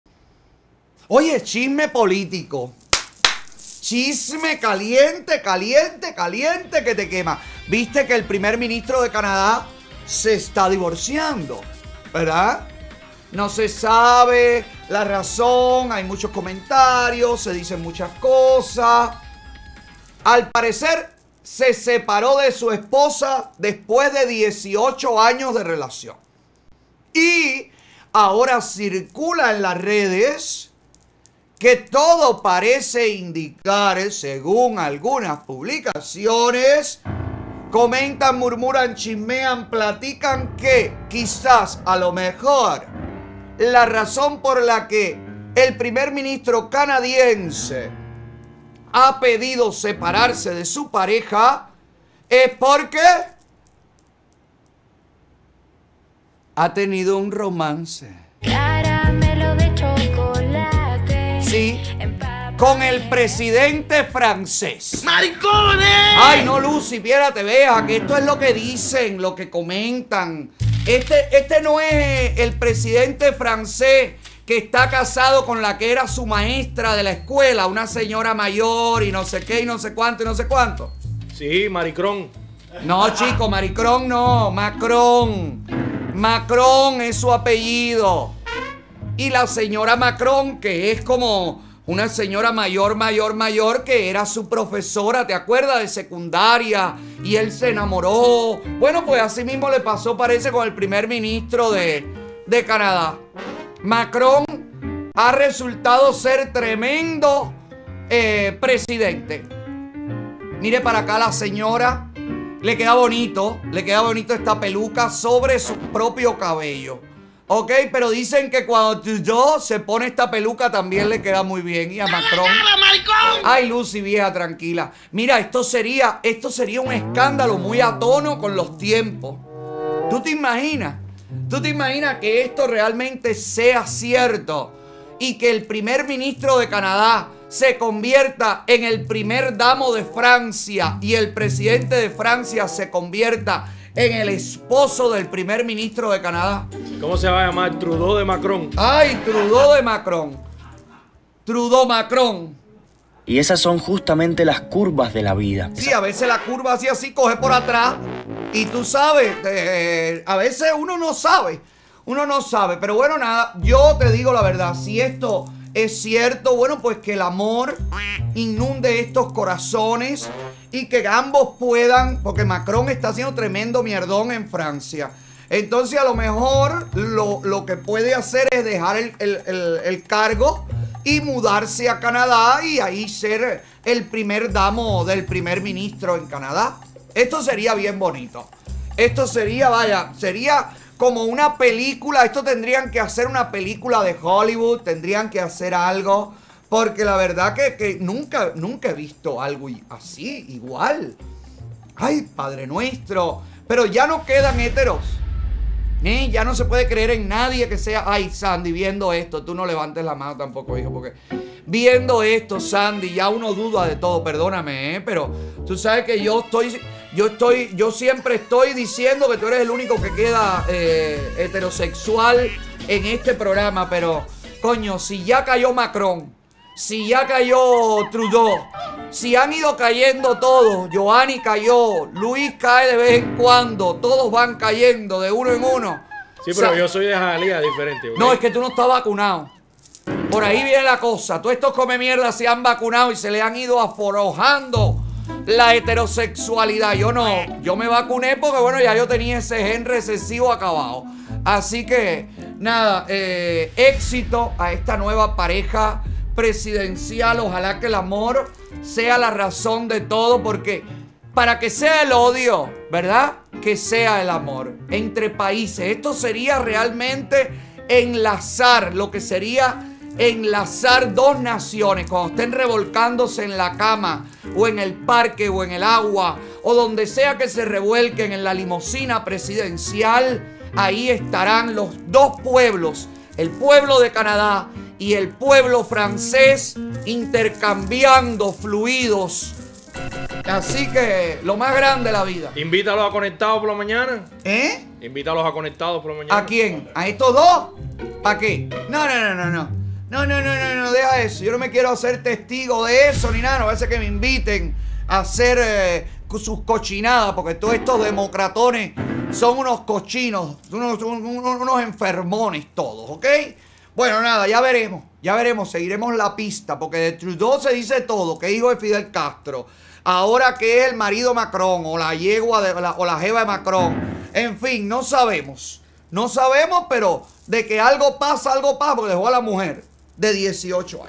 Otaola califica escándalo muy a tono con los tiempos”, posible romance entre Trudeau y Macron
El Rey del Chisme califica como “Un escándalo muy a tono con los tiempos”, un posible romance entre Justin Trudeau y Emmanuel Macron. “Esto sería enlazar dos naciones”.